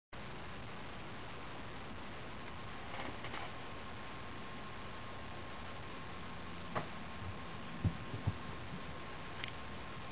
BasementCat.wmv
Poor Karma Minding her own business when BASEMENT CAT ATTACKS!